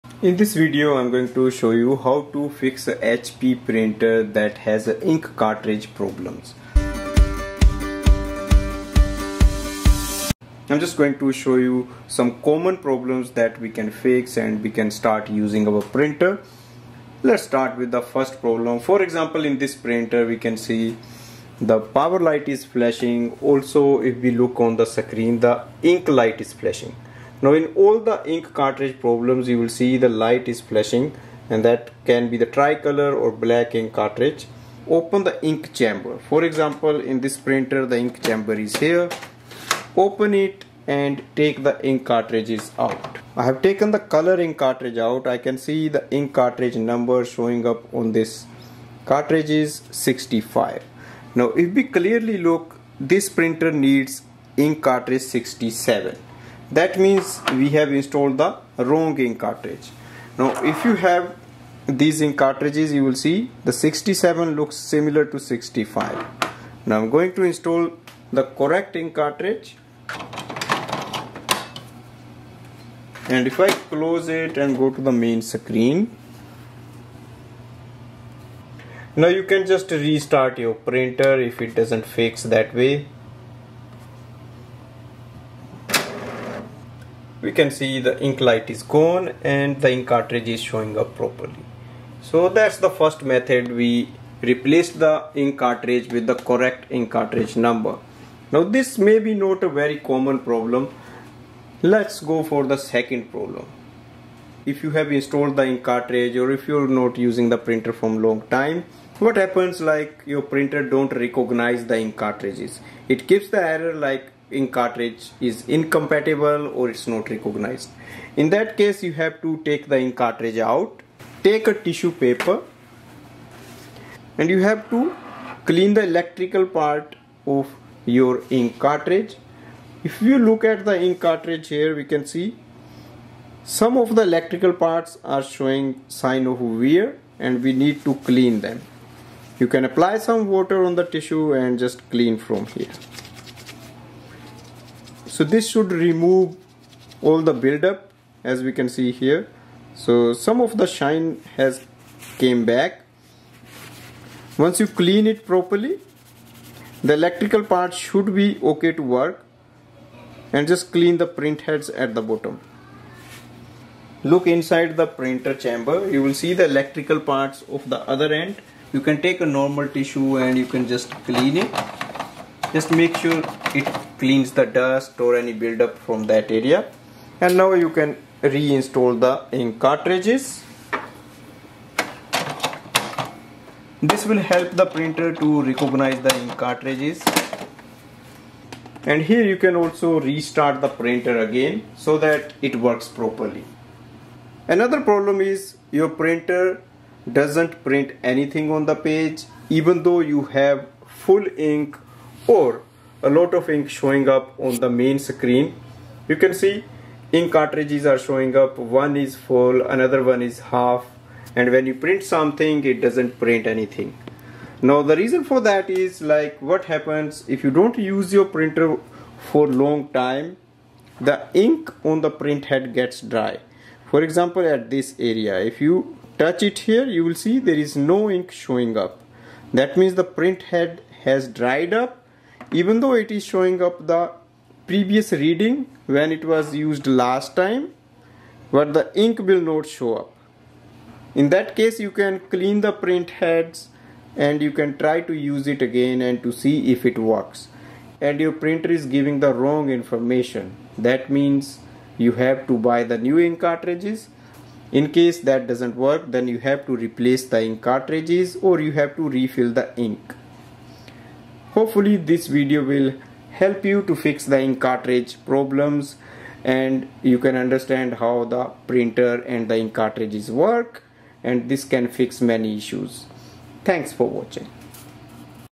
HP Printer Ink Cartridge Problems !
This video reviews how to fix ink cartridge errors or problems in any HP Printer.